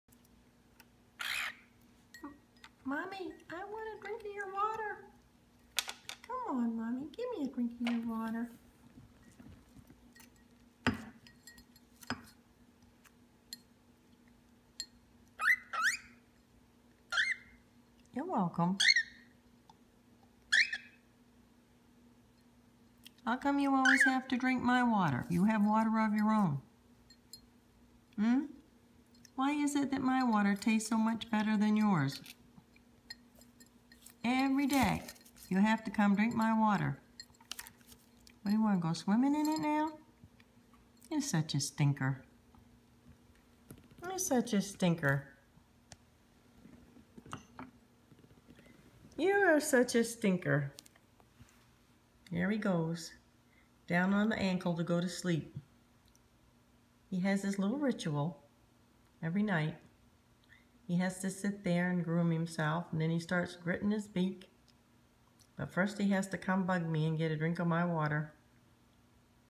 Moody cockatiel asks for mommy’s water
Back when she was a chick and we though she was a he